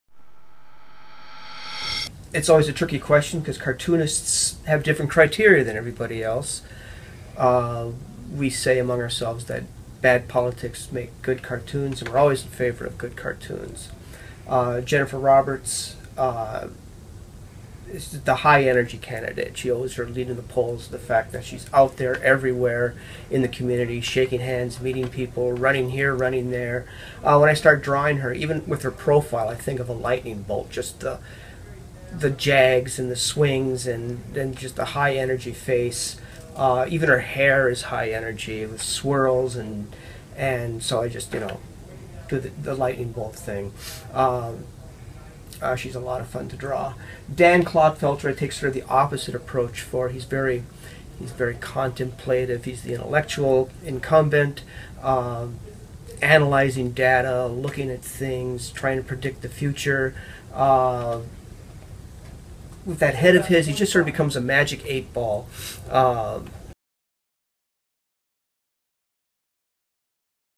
Behind the sketches with Kevin Siers
Pulitzer Prize winning cartoonist Kevin Siers shows how he sketches local politicians.